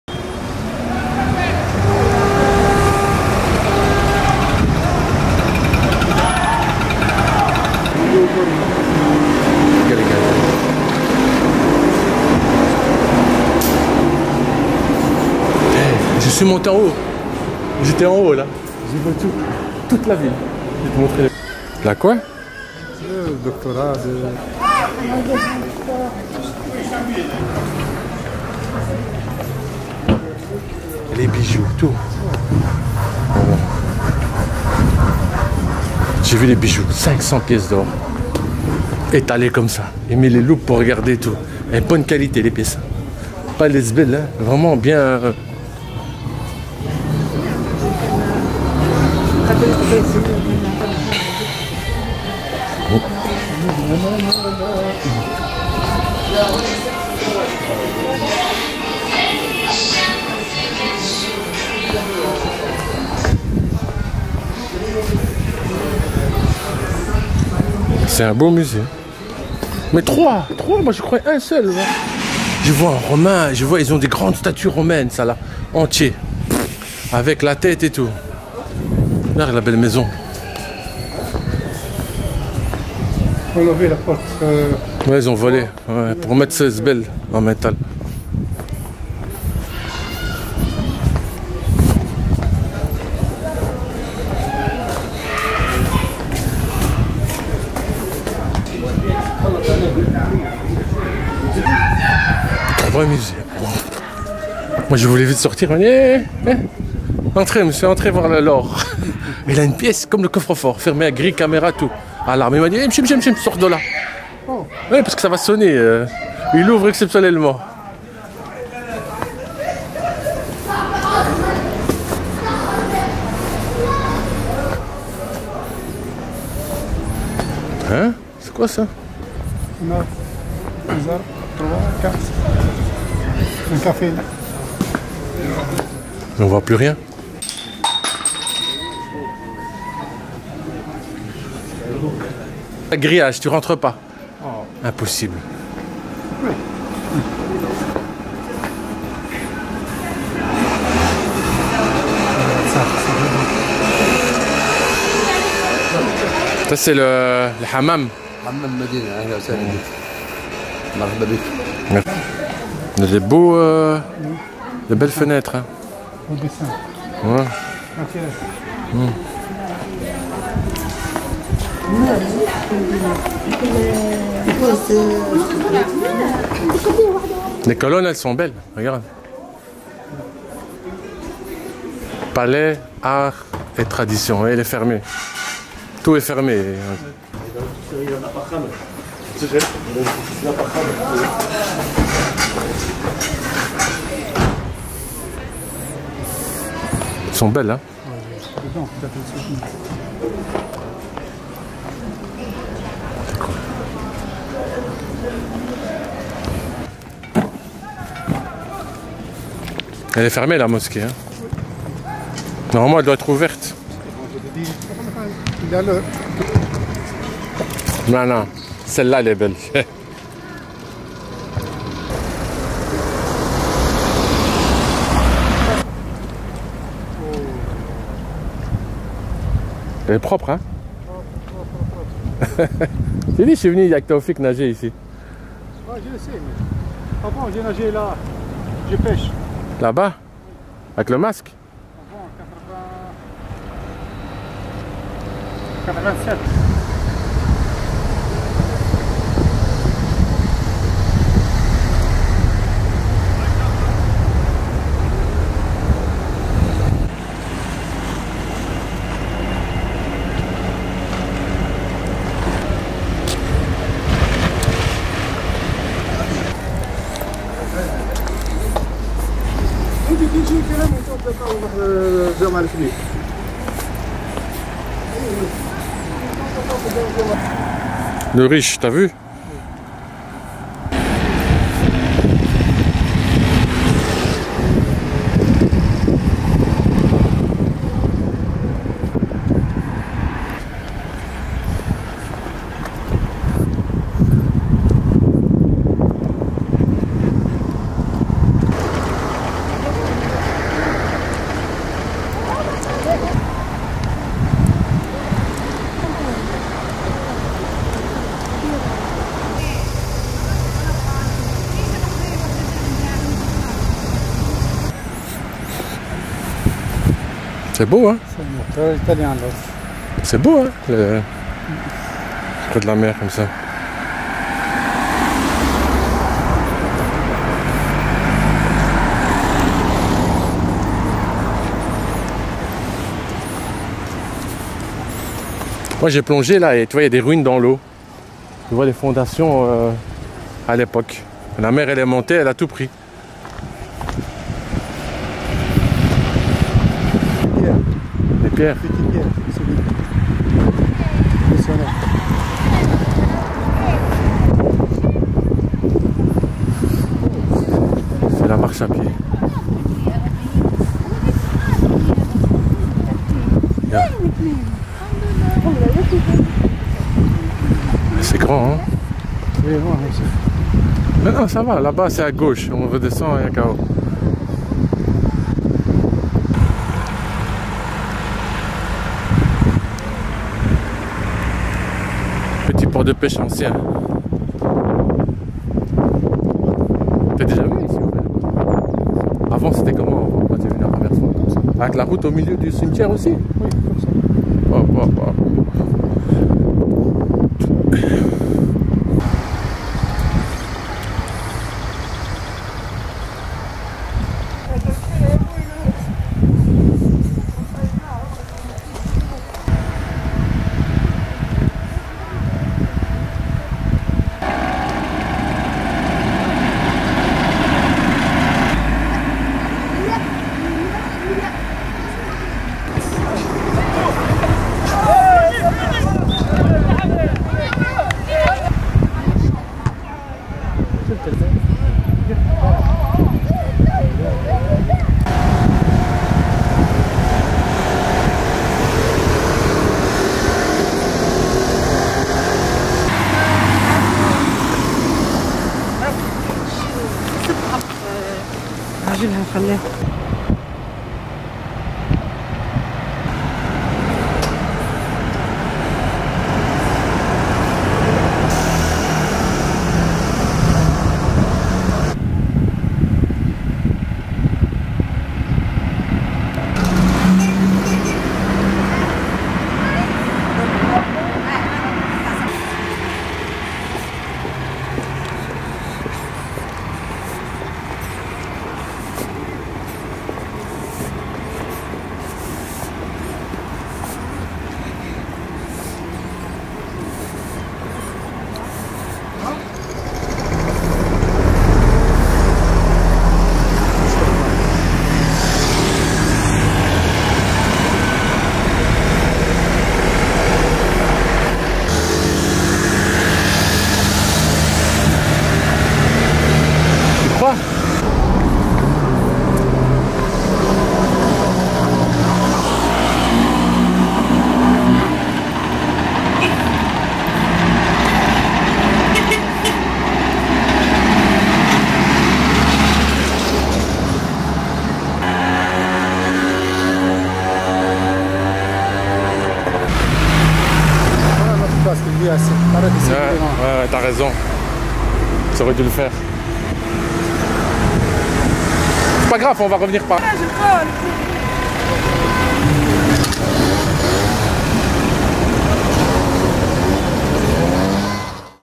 Présentation du circuit touristique de Mahdia
Quand ont connaît Mahdia ont prend le chemin du vieux quartier par ce que c'est la qu'il faut aller pour se promener. On a une belle vue sur la mer, et on passe devant le vieux cimetière. Et puis on peut aller aussi dans la médina regarder les petites boutiques de souvenir. On trouve deux cafés avec des tables sous les arbres. Ça donne un beau charme dans le lieu. En tout cas on ne s’ennuie pas.